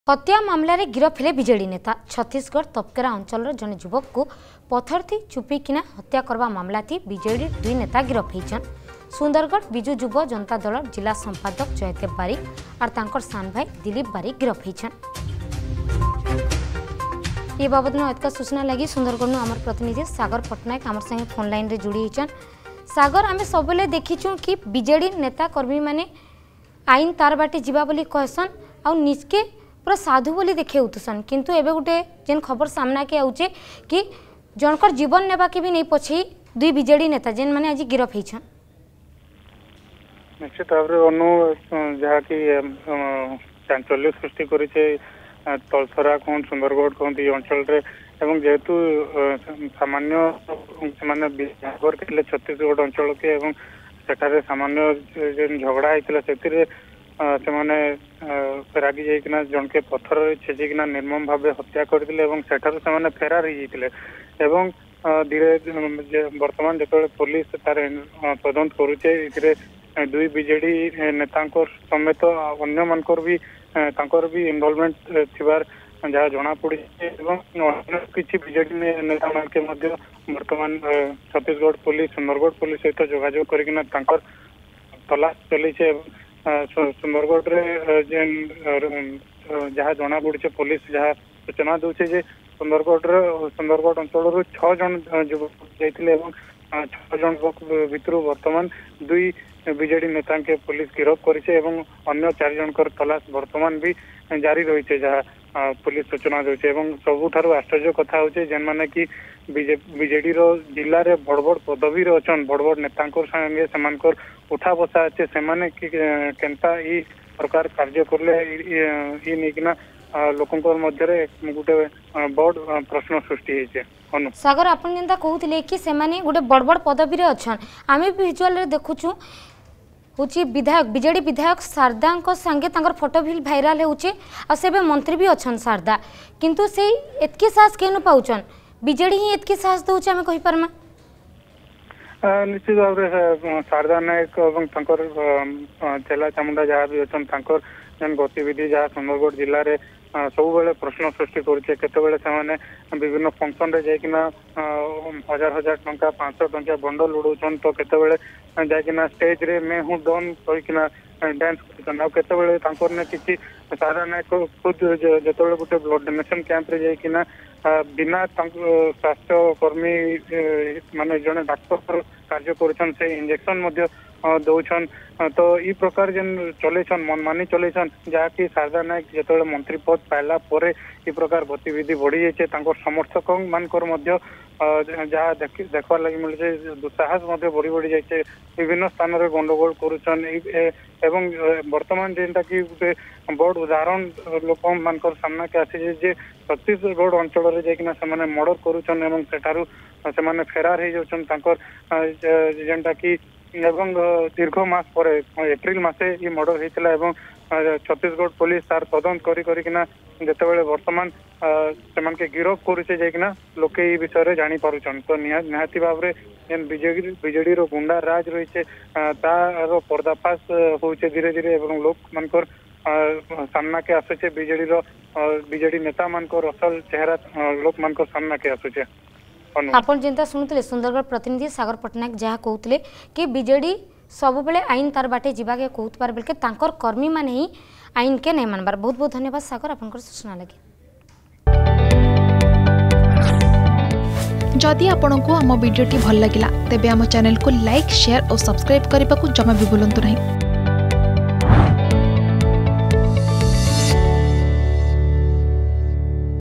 ହତ୍ୟା ମାମଲାରେ ଗିରଫ୍ ବିଜେଡି ନେତା || BJD leader arrested
ବିଜେଡି ବିଧାୟକ ଶାରଦା ନାୟକକଁର୍ ସଙ୍ଗେ ଅଭିଯୁକ୍ତକଁର୍ ଫଟୋ ଭାଏରାଲ୍ । ଛତିଶଗଡ଼ ତପକେରା ଅଞ୍ଚଲର୍ ଝନେ ଯୁଆନକେ ପଥର୍ ଚୁପିକି ମର୍ଡର୍ ଘଟନା । ସୁନ୍ଦରଗଡ ବିଜେଡିର୍ ଜିଲ୍ଲା ସମ୍ପାଦକ ଜୟଦେବ ବାରିକ ଆର୍ ତାକଁର୍ ସାନ ଭାଏ ଦିଲିପ ।